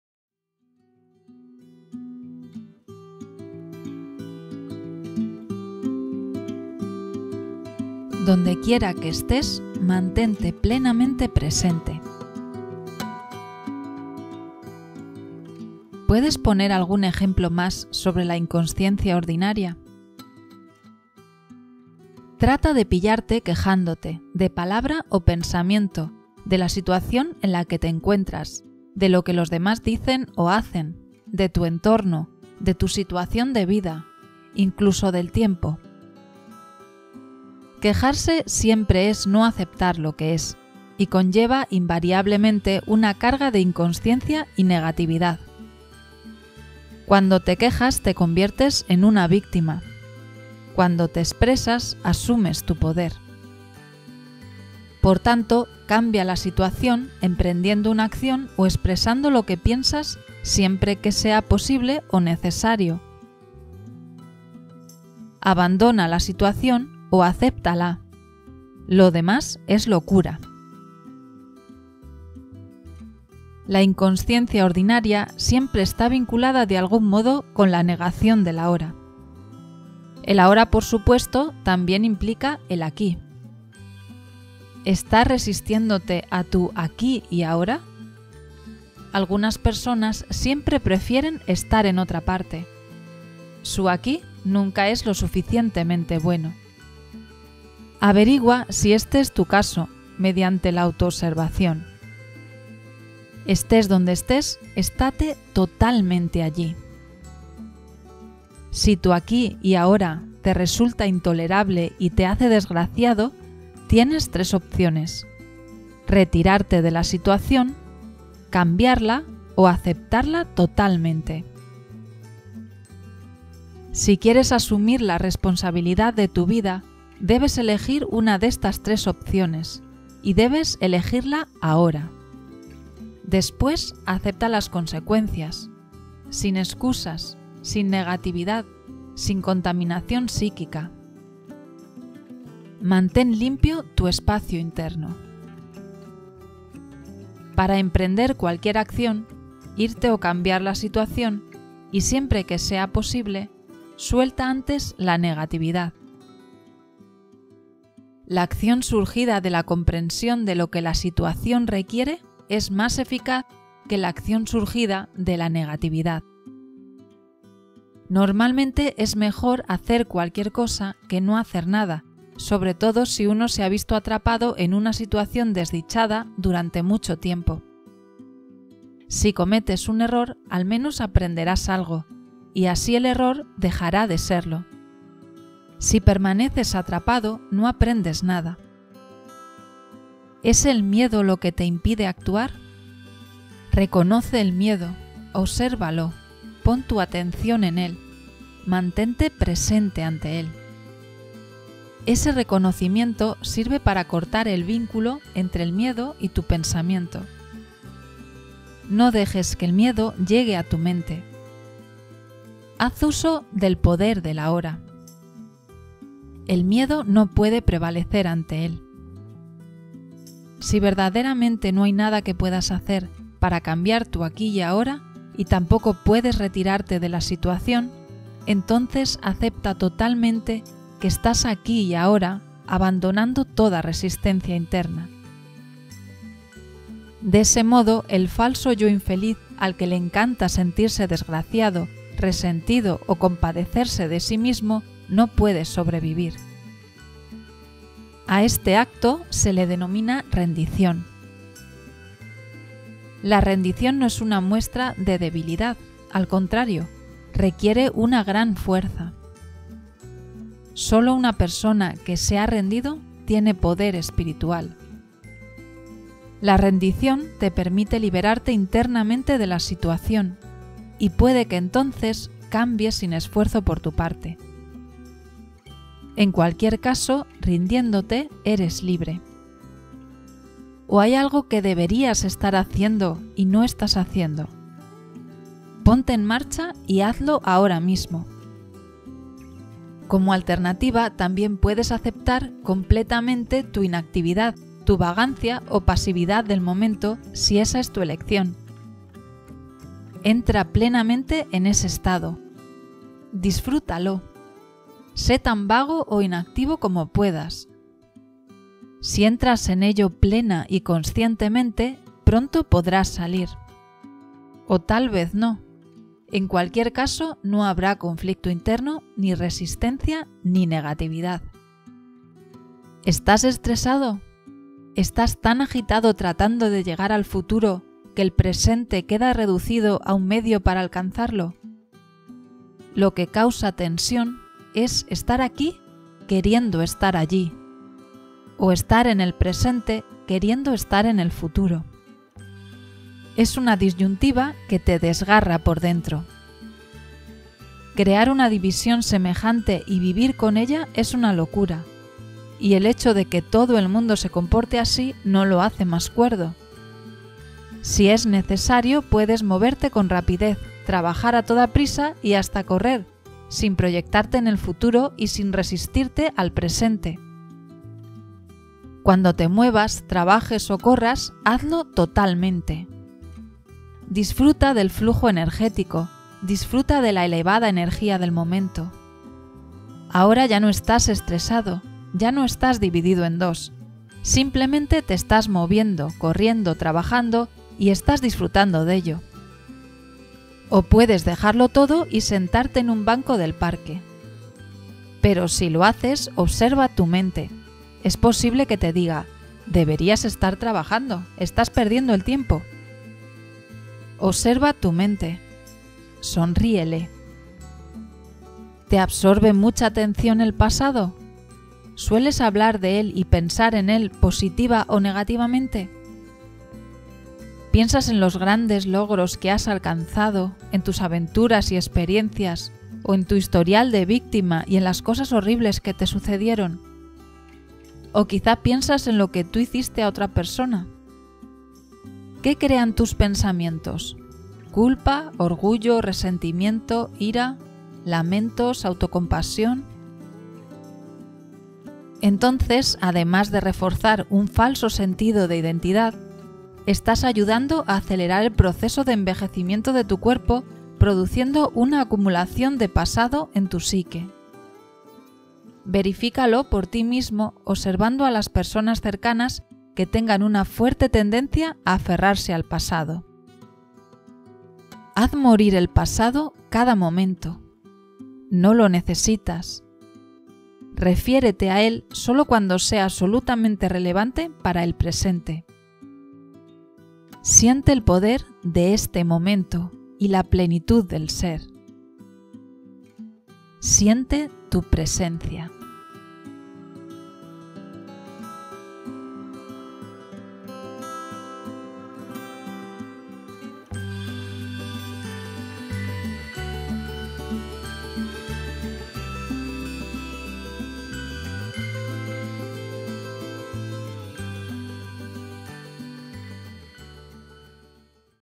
Donde quiera que estés mantente plenamente PRESENTE - Eckhart Tolle
- ¿Puedes poner algún ejemplo más sobre la inconsciencia ordinaria?
- Trata de pillarte quejándote, de palabra o pensamiento, de la situación en la que te encuentras, de lo que los demás dicen o hacen, de tu entorno, de tu situación de vida, incluso del tiempo. Quejarse siempre es  no aceptar lo que es, y conlleva inavariablemente una carga de inconscienca y negatividad. Cuando te quejas, te conviertes en una víctima; cuando te expresas, asumes tu poder...